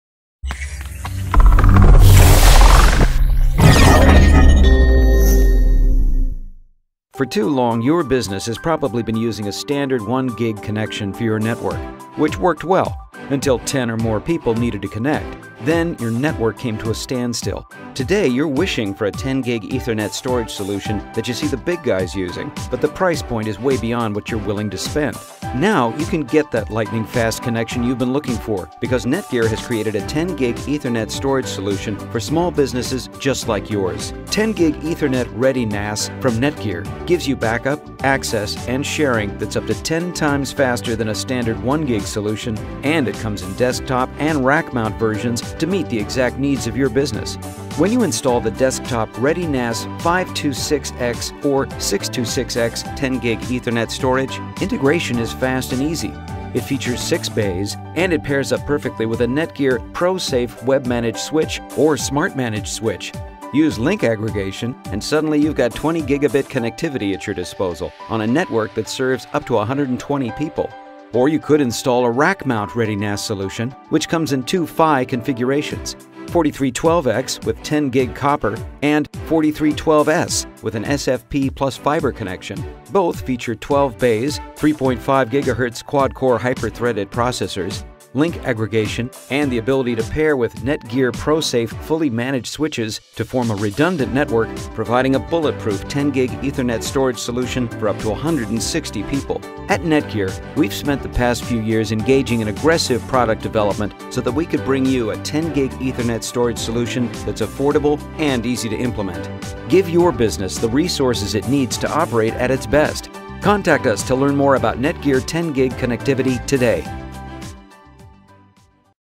NETGEAR 中小企業最強10 GbE ReadyNAS 網路儲存伺服器
NETGEAR 全新系列「網路儲存服器」ReadyNAS 強效登場!

您的工作場所總是遇到NAS存取緩慢的問題嗎?
擁有數十人的工作團隊，確還在共享GbE等級的網路儲存頻寬嗎?
NETGEAR 全新的ReadyNAS 讓你無痛升級至10 GbE 的存取環境，完整提升文件檔案的存取、備份與保護功能，增進工作效率!